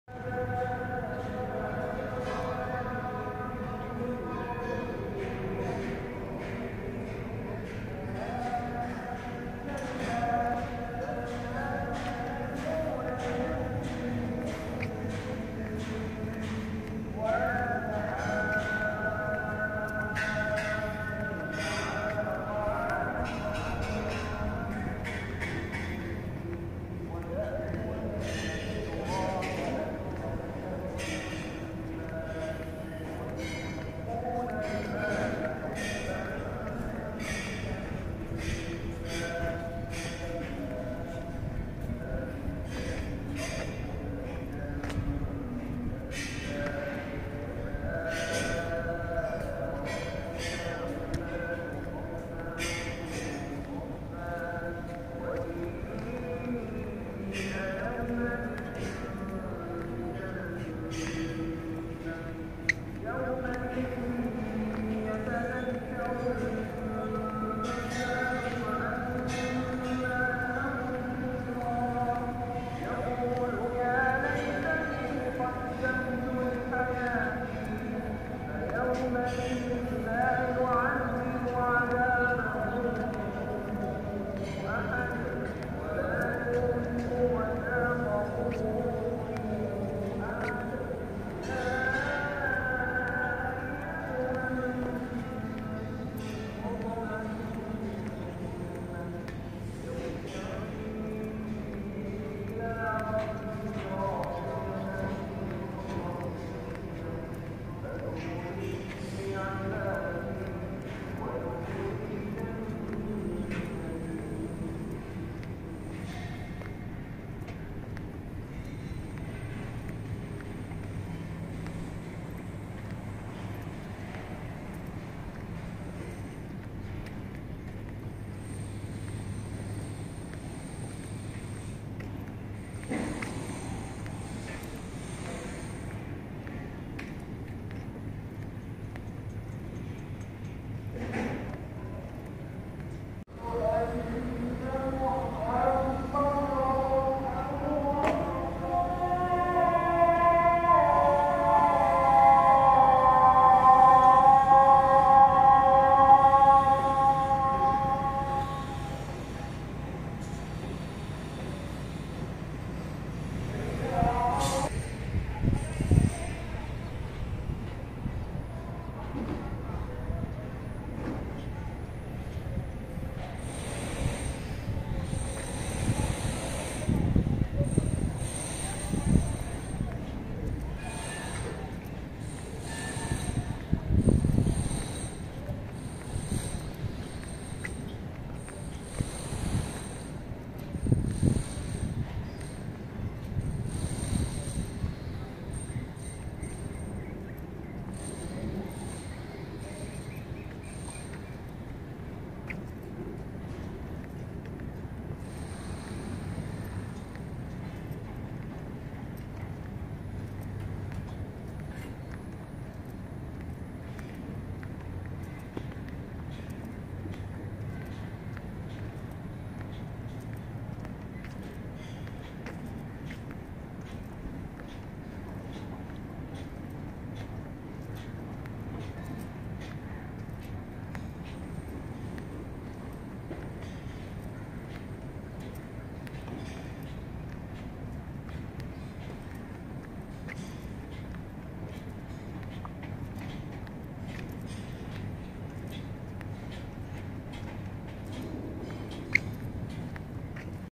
Putrajaya Lake 2023
I was here year 2010. I don't walk much because it was so hot here. I always amazed by the man- made lake and the bridges